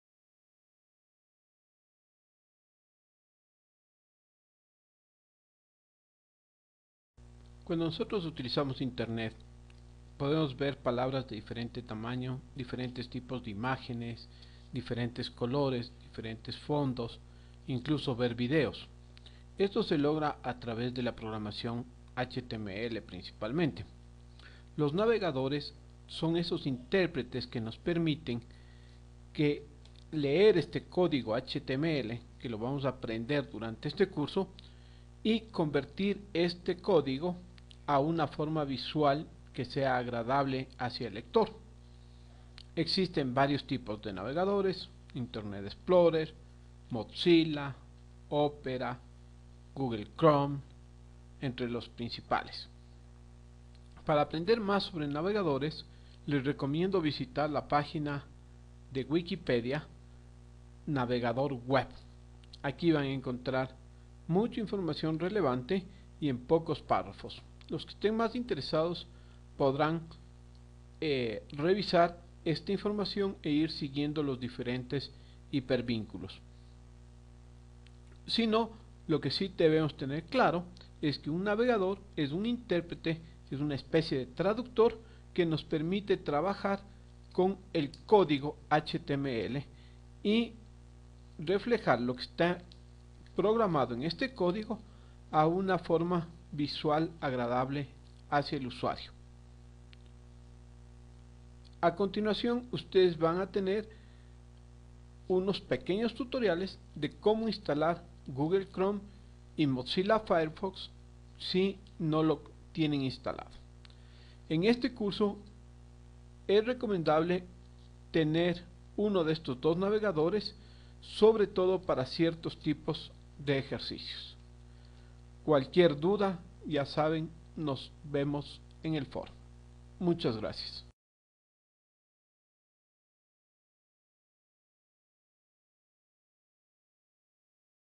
navegadores
Explicación de que es una navegador web dentro del curso programación html